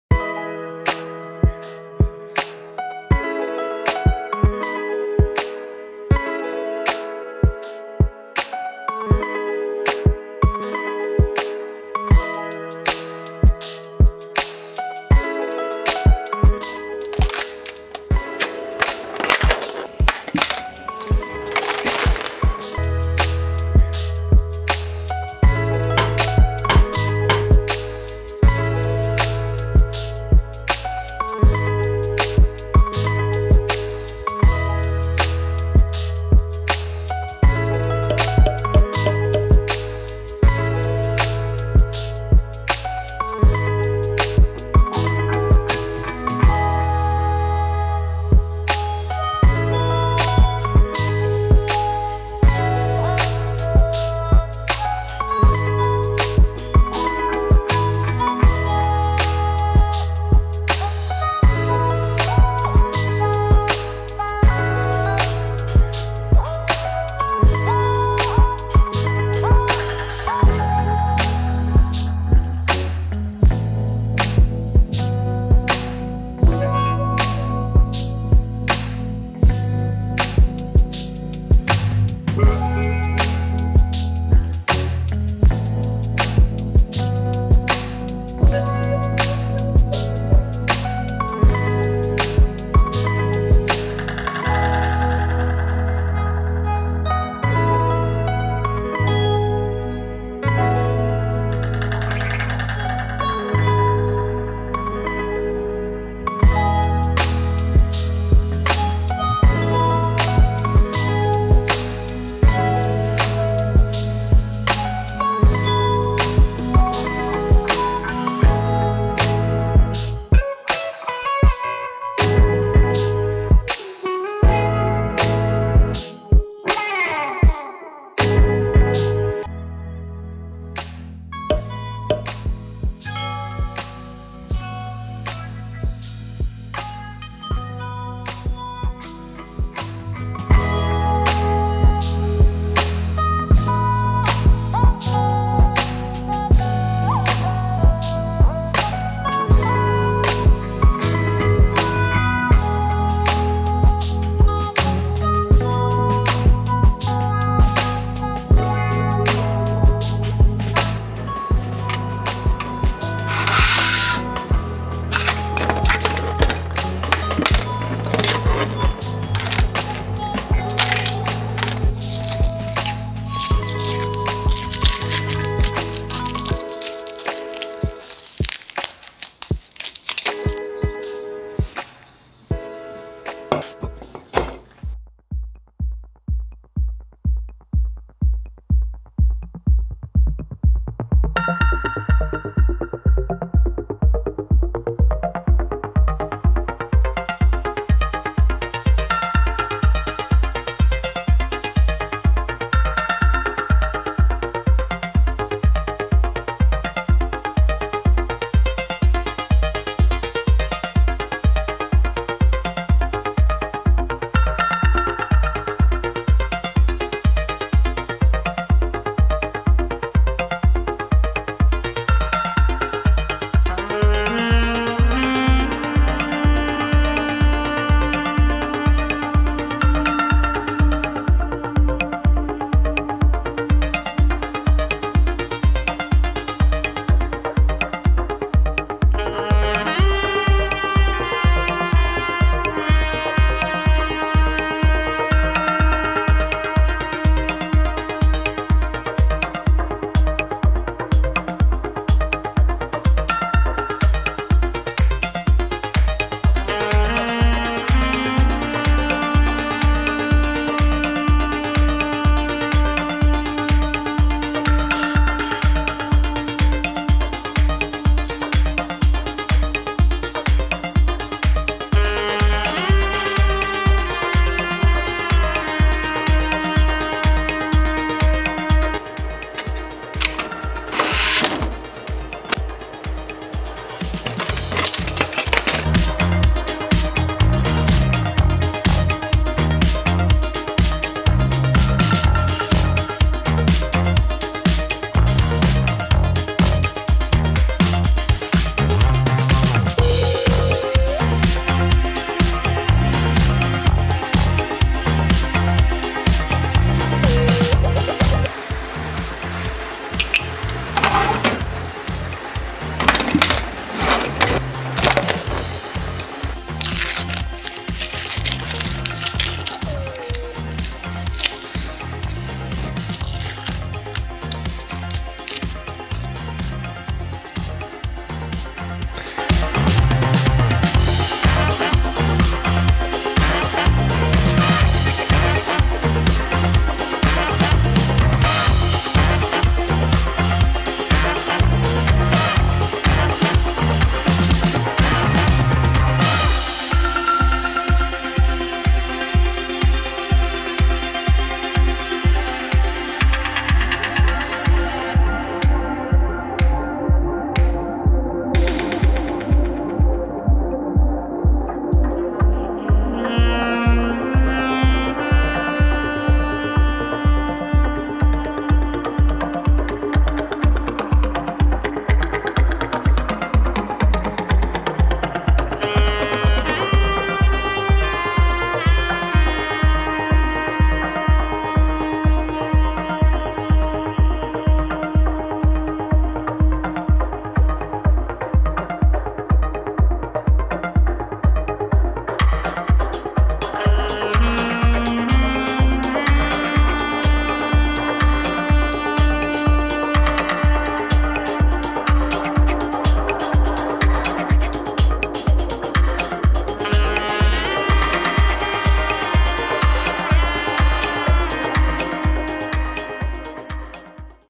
Modular Starfield Ship Model 3D Printed
Thank you Qidi for supplying the Tech X-Plus 3 for my video! Fantastic 3D printer that's capable of creating high quality prints at warp speed.